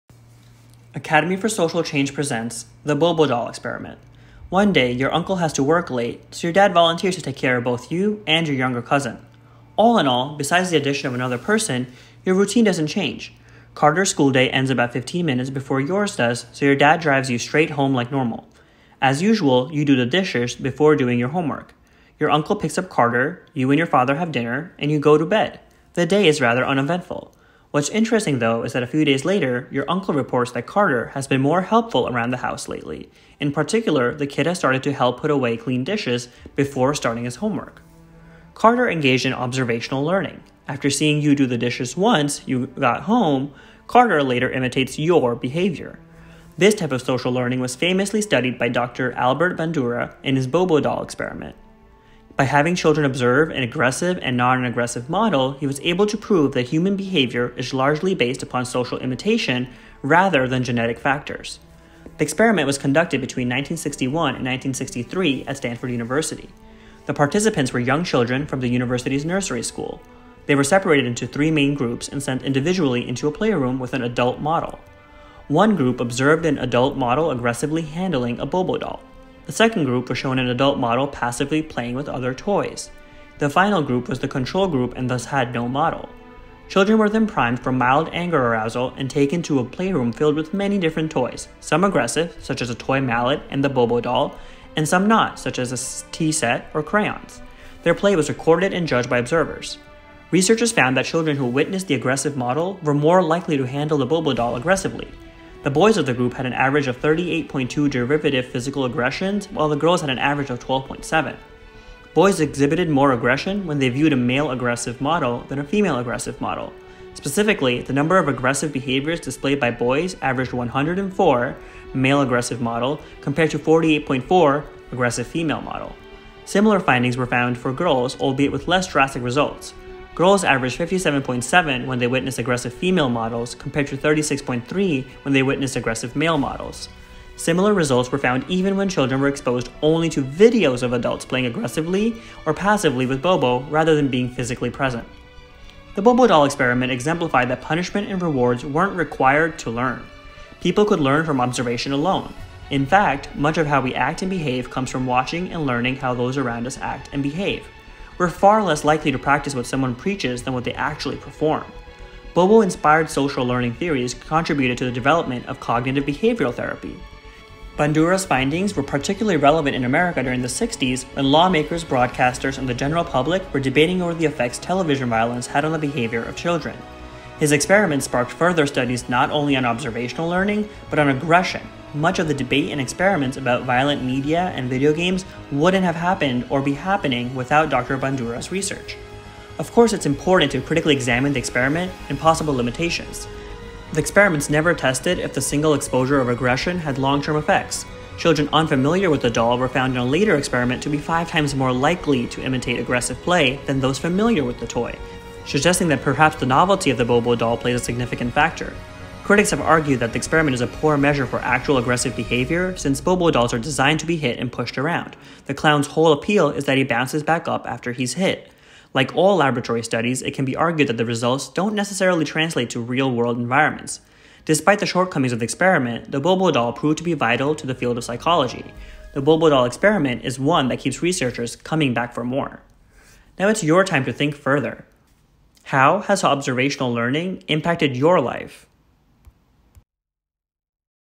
Bobo Doll Experiment: Kids See, Kids Do - Psychology Experiments | Academy 4 Social Change
This type of social learning was famously studied by Dr. Albert Bandura in his Bobo Doll Experiment. By having children observe an aggressive and nonaggressive model, he was able to prove that human behavior is largely based upon social imitation rather than genetic factors.

**Think Further Questions:
1. What do you think is the most important finding of the Bobo Doll Experiment?
2. How has observational learning impacted your life?
3. What are some other limitations of the Bobo Doll Experiment?

**Contents
00:00 - Have You Ever?
00:38 - Here’s Why
00:46 - Bobo Doll Experiment
01:02 - The Experiment
20:25 - Why Care?